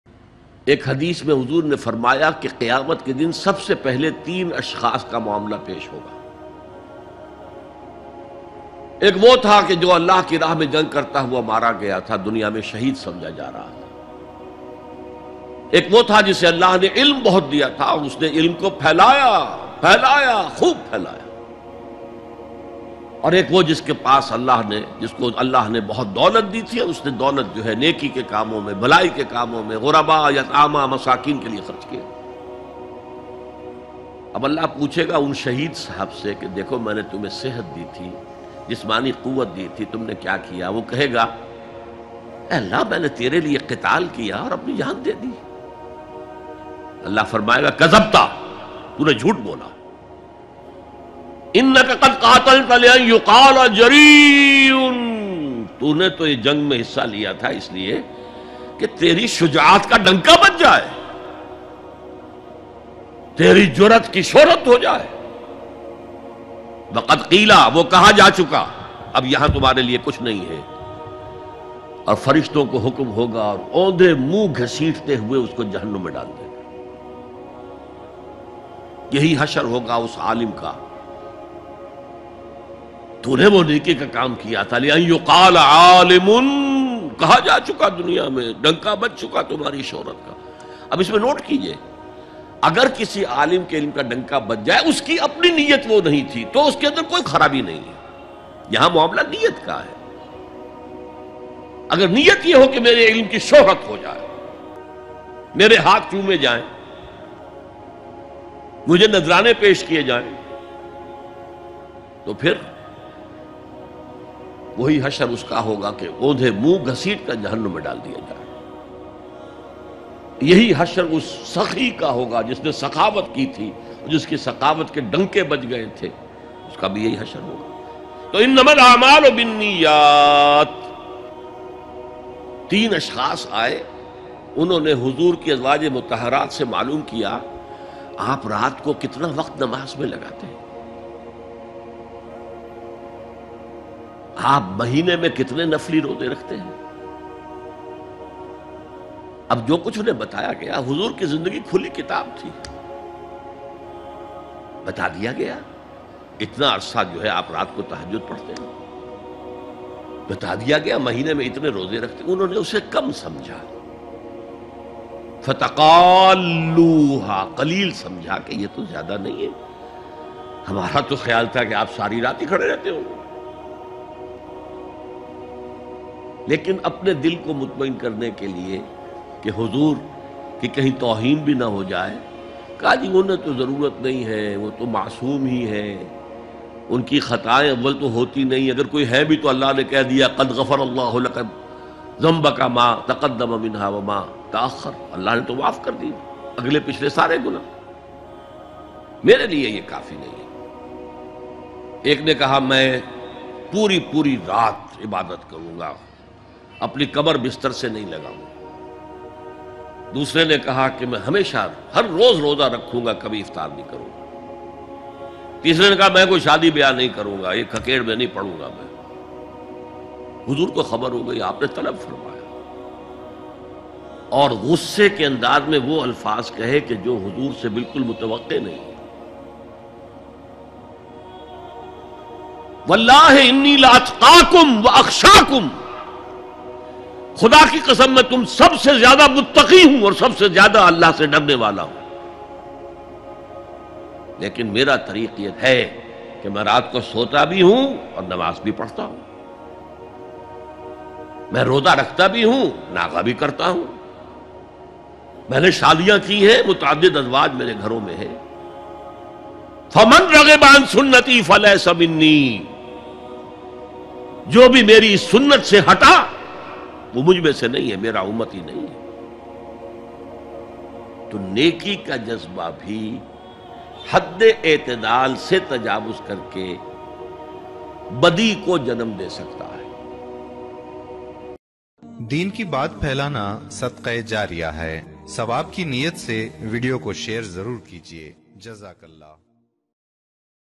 Ankhain khol dainay walay 2 waqiat by Dr Israr Ahmed
لوگوں کو دکھانے کے لئے نیکی کرنا
دنیا کے معاملات چھوڑ کر صرف عبادت میں لگ جانا